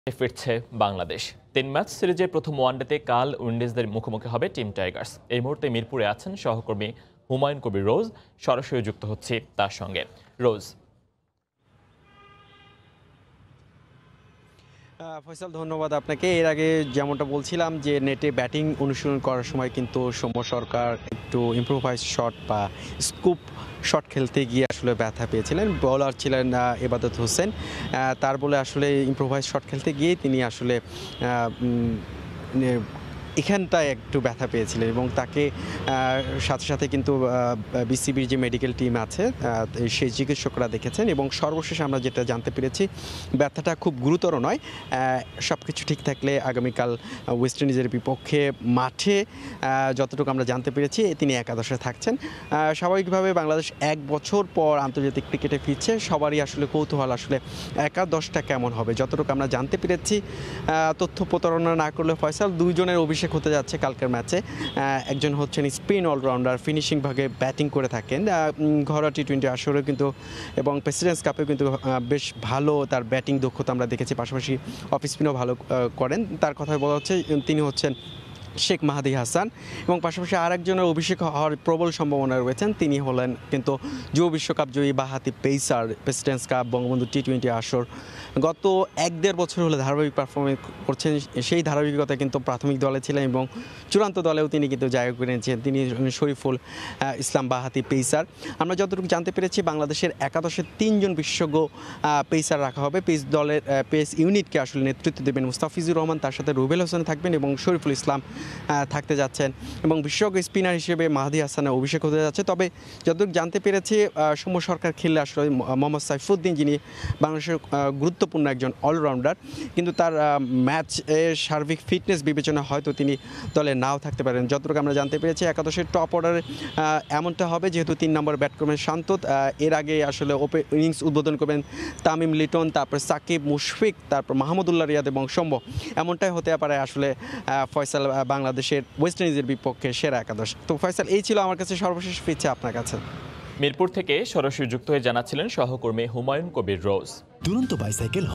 আবারো আন্তর্জাতিক ক্রিকেটে ফিরছে বাংলাদেশ | Sports News
৩১২ দিনের অপেক্ষার সমাপ্তি হচ্ছে। আবারো আন্তর্জাতিক ক্রিকেটে ফিরছে বাংলাদেশ। তিন ম্যাচ সিরিজের প্রথম ওয়ানডেতে, কাল উইন্ডিজদের মুখোমুখি হবে টিম টাইগার্স।